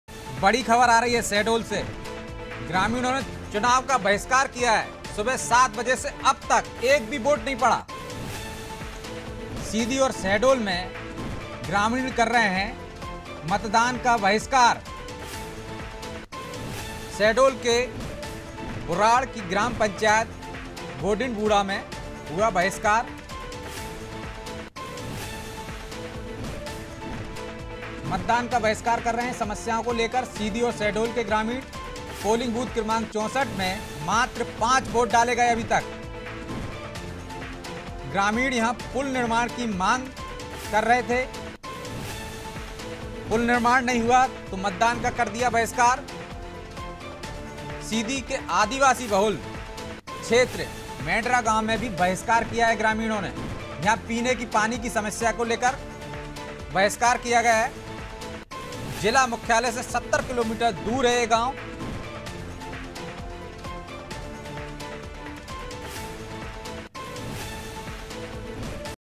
Shahdol : सीधी और शहडोल में ग्रामीणों ने किया चुनाव का बहिष्कार | Election Breaking || Anaadi Tv
शहडोल : ग्रामीणों ने किया चुनाव का बहिष्कार
सुबह 7 बजे से नहीं पड़ा एक भी वोट
सीधी और शहडोल में ग्रामीणों ने किया बहिष्कार
शहडोल के बुढ़ार की ग्राम पंचायत गोडिन बूडा में बहिष्कार
पोलिंग बूथ क्रमांक 64 में मात्र 5 वोट ही डाले गए हैं
ग्रामीणों ने पुल निर्माण की मांग को लेकर किया बहिष्कार
सीधी के आदिवासी बाहुल्य क्षेत्र मेडरा गांव में बहिष्कार
पीने के पानी की समस्या को लेकर बहिष्कार
जिला मुख्यालय से 70 ​किलोमीटर दूर है मेडरा गांव

AIRTEL - 379